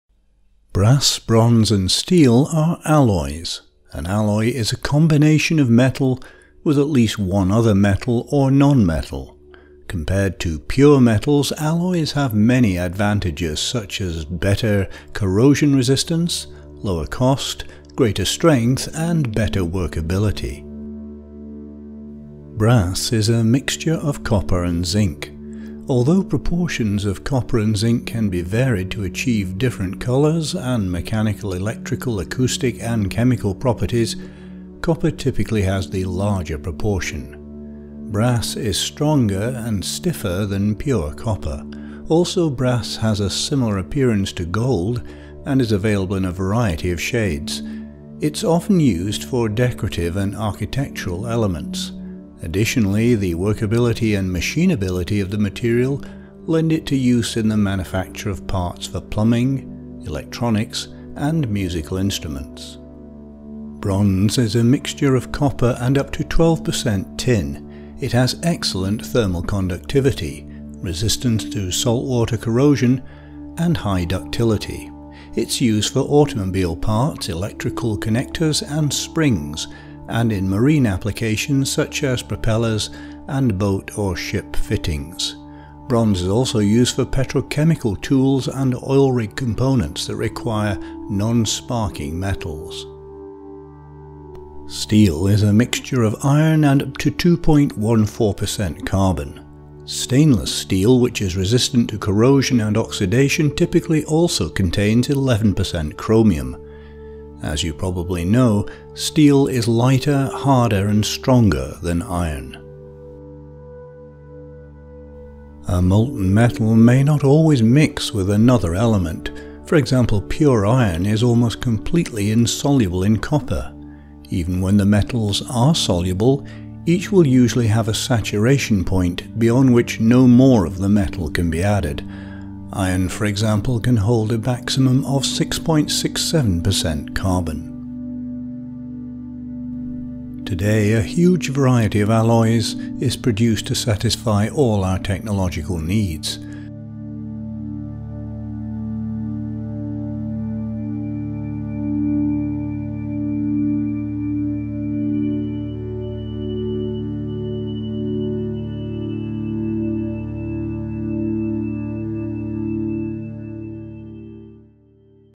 Brass, Bronze, Steel
📚𝐃𝐚𝐯𝐢𝐝'𝐬 𝐁𝐨𝐨𝐤𝐬
📙 𝗪𝗲𝗶𝗿𝗱𝗲𝗿 𝗠𝗮𝘁𝗵𝘀: 𝗔𝘁 𝘁𝗵𝗲 𝗘𝗱𝗴𝗲 𝗼𝗳 𝘁𝗵𝗲 𝗣𝗼𝘀𝘀𝗶𝗯𝗹𝗲
📗 𝗪𝗲𝗶𝗿𝗱𝗲𝘀𝘁 𝗠𝗮𝘁𝗵𝘀: 𝗔𝘁 𝘁𝗵𝗲 𝗙𝗿𝗼𝗻𝘁𝗶𝗲𝗿𝘀 𝗼𝗳 𝗥𝗲𝗮𝘀𝗼𝗻

📄𝗧𝗿𝗮𝗻𝘀𝗰𝗿𝗶𝗽𝘁𝗶𝗼𝗻:
Brass, bronze and steel are alloys. An alloy is a combination of a metal with at least one other metal or nonmetal. Compared to pure metals, alloys have many advantages such as better corrosion resistance, lower cost, greater strength and better workability.

Brass is mixture of copper and zinc. Although proportions of copper and zinc can be varied to achieve different colors, and mechanical, electrical, acoustic, and chemical properties, copper typically has the larger proportion. Brass is stronger and stiffer than pure copper. Also brass has a similar appearance to gold and is available in a variety of shades. It’s often used for decorative and architectural elements. Additionally, the workability and machinability of the material lend it to use in the manufacture of parts for plumbing, electronics, and musical instruments.

Bronze is mixture of copper and up to 12% tin. It has excellent thermal conductivity, resistance to saltwater corrosion, and High ductility. It’s used for automobile parts, electrical connectors and springs, and in marine applications, such as propellers and boat or ship fittings. Bronze is also used for petrochemical tools and oil rig components that require non-sparking metals.

Steel is mixture of iron and up to 2.14% carbon. Stainless steel, which is resistant to corrosion and oxidation, typically also contains 11% chromium. As you know probably know, steel is lighter, harder and stronger than iron.

A molten metal may not always mix with another element. For example, pure iron is almost completely insoluble in copper. Even when the metals are soluble, each will usually have a saturation point, beyond which no more of the metal can be added. Iron, for example, can hold a maximum of 6.67% carbon.

Today, a huge variety of alloys is produced to satisfy all our technological needs. For example in the case of bronze, aluminum bronze has good abrasion resistance, nickel aluminum bronze is highly great flexible, tin bronze is highly castable, manganese bronze is very strong, and lead tin bronze can be made at low cost.